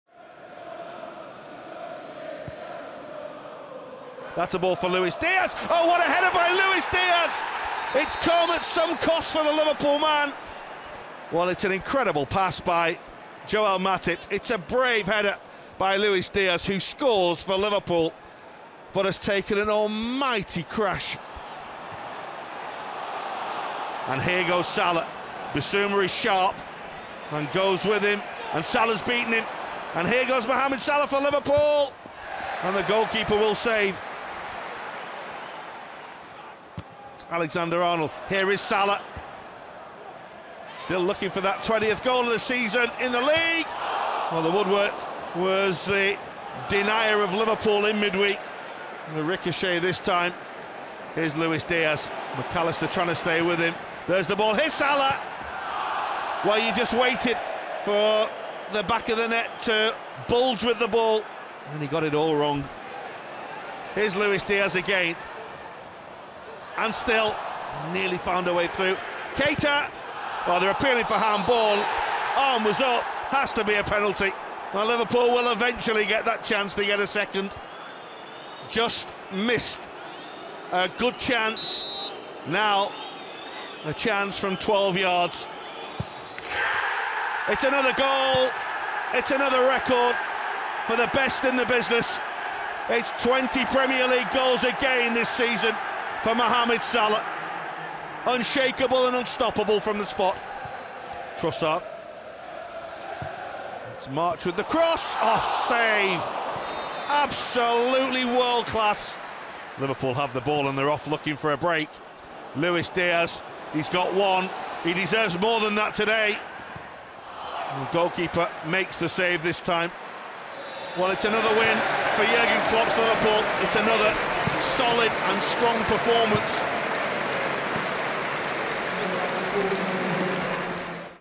Highlights: Brighton & Hove Albion 0-2 Liverpool | Diaz & Salah fire Reds to victory on south coast
Get full-match replays, exclusive training access and so much more on LFCTV GO. Use the code 2122GOFREE to get your first month FREE!